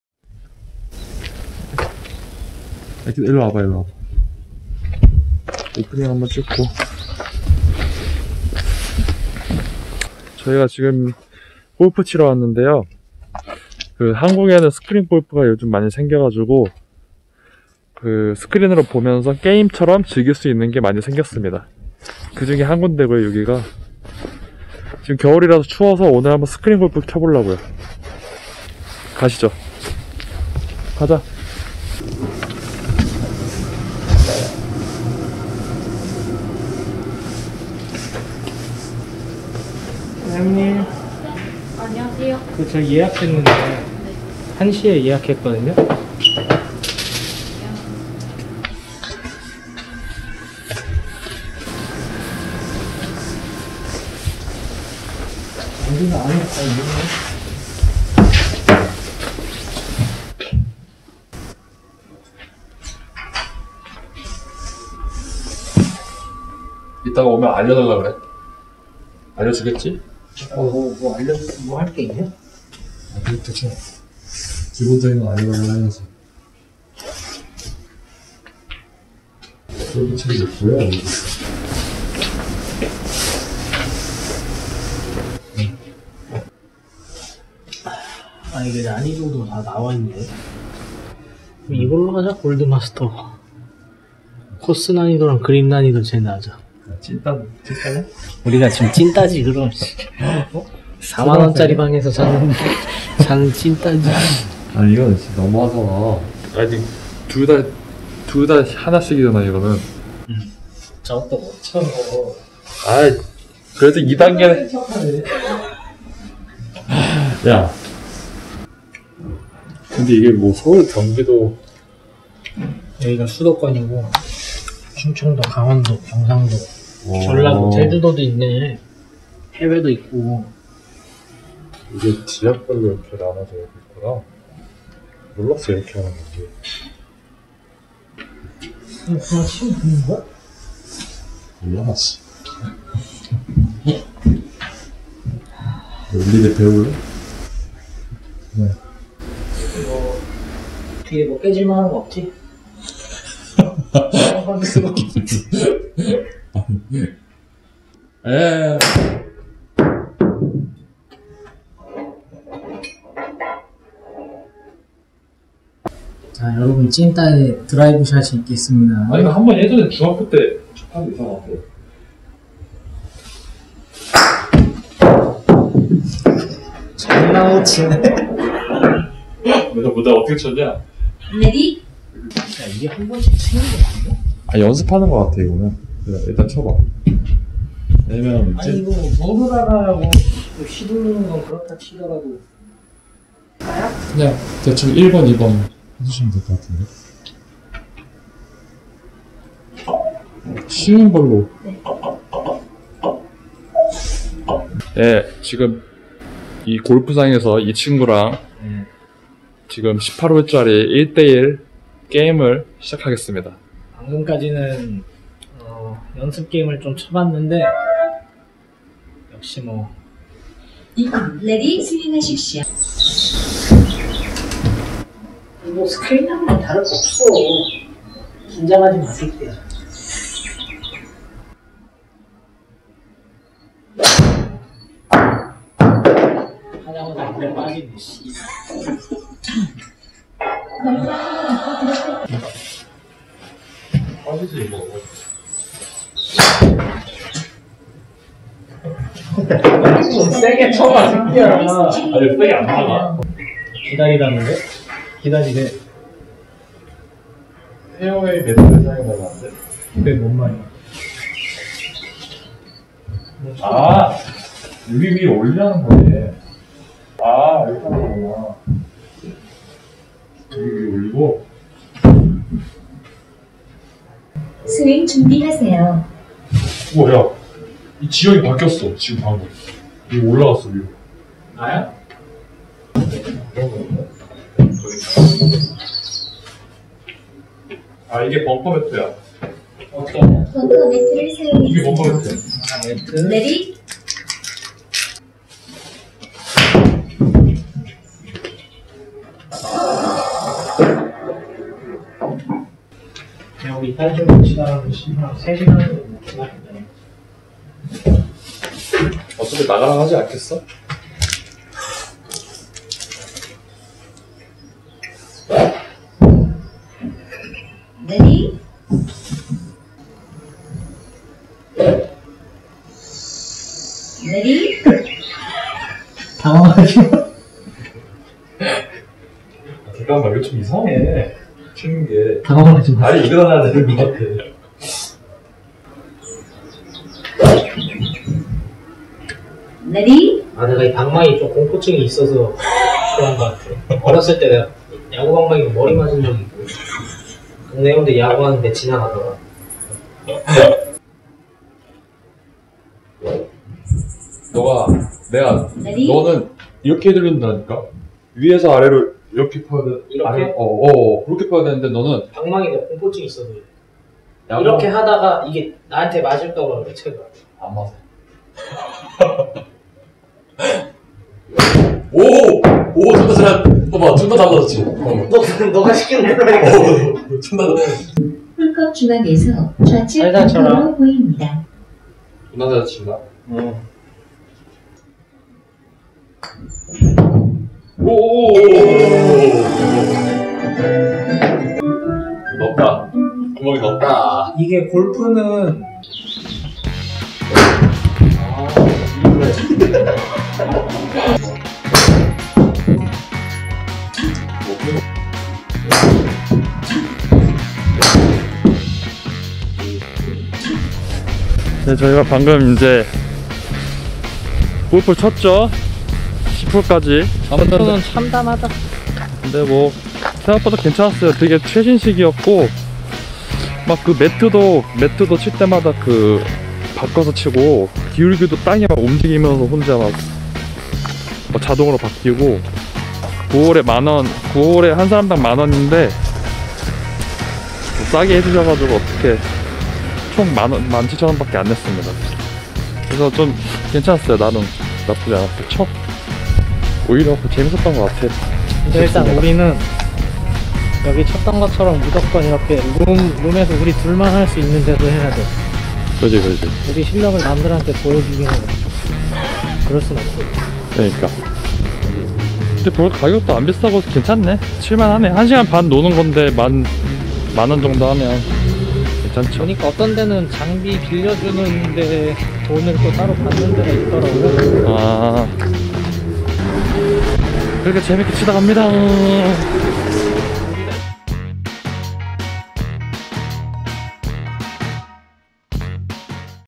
스크린 골프 처음 쳐봤습니다.
한국에서 스크린 골프가 유행하고 있는데요. 왜 그런지 궁금해서 한 번 쳐봤습니다.

00:00 오프닝
00:32 스크린 골프장 모습
02:19 골프 시작
06:52 리뷰

"전세계의 문화적 장벽을 허물겠습니다"